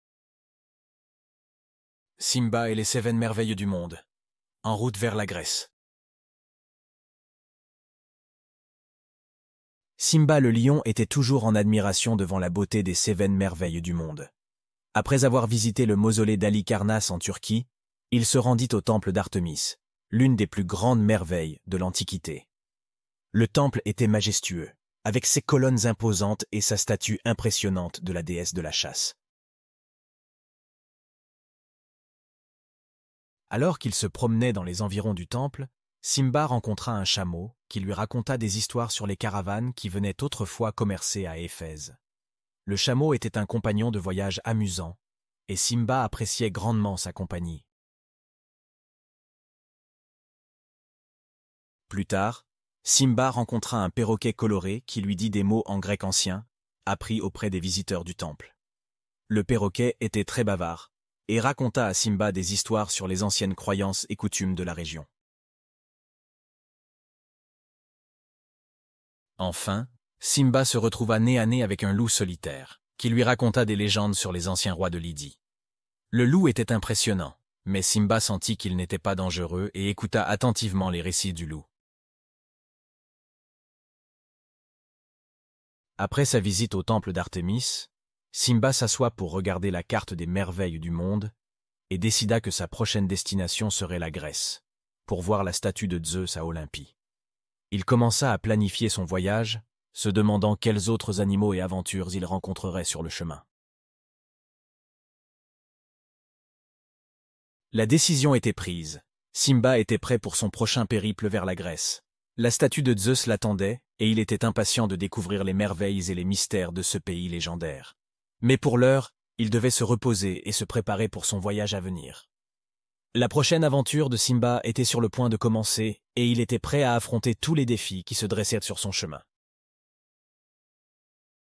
[4/7] Simba et les 7 merveilles du monde En route vers la Grèce | Conte pour enfants
📖 Simba le lion admirait les 7 merveilles du monde, visitant le temple d'Artemis en Turquie où il rencontra un chameau, un perroquet et un loup qui lui racontèrent des histoires fascinantes avant de décider de partir pour la Grèce pour voir la statue de Zeus à Olympie.